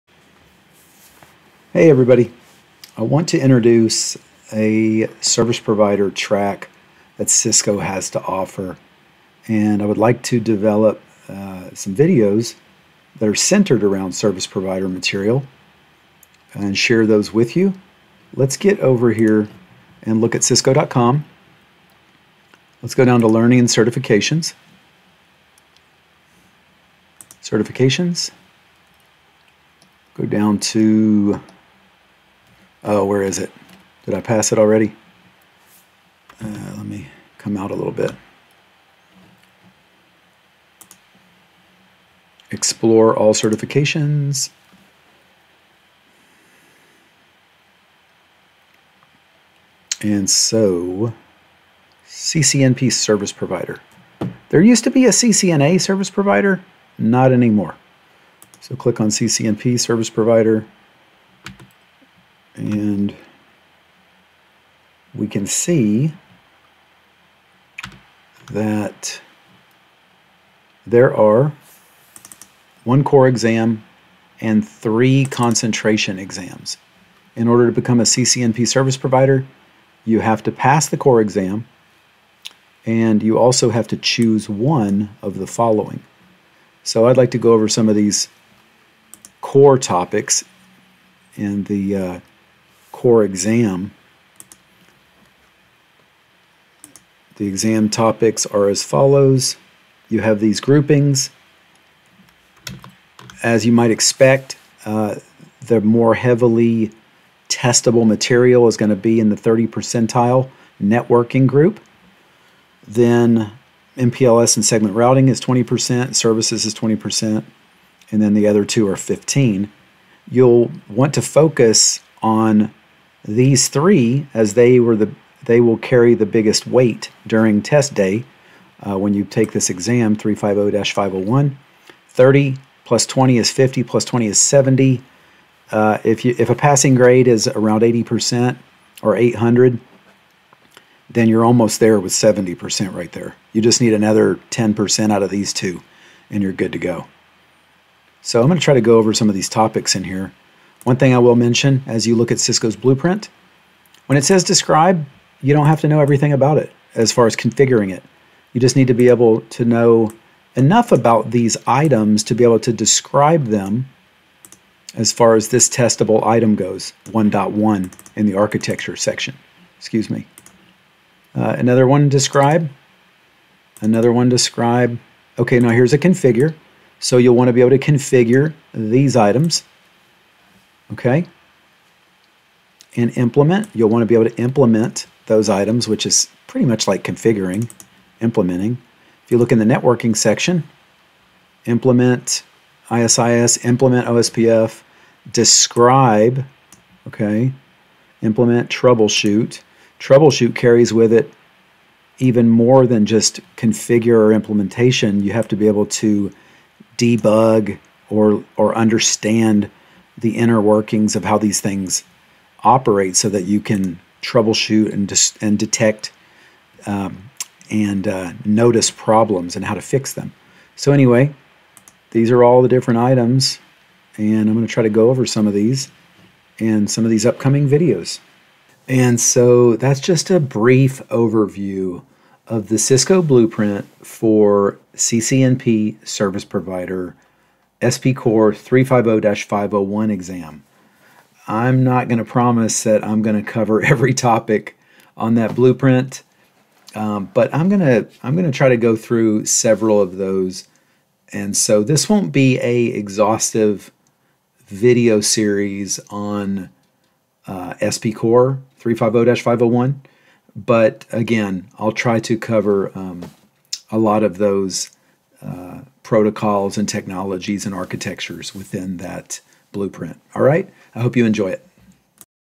1 - spcor - 350-501 - intro
let's briefly look over the cisco ccnp sp blueprint

I like this link that shows Cisco's Certification Paths/Tracks...